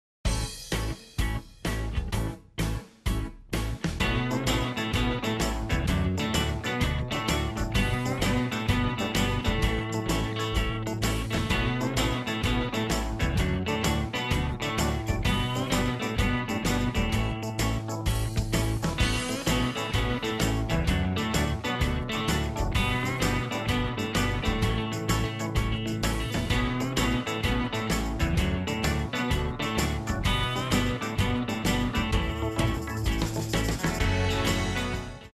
Patent Jacket Red Boden
british commercial promoting a red rain jacket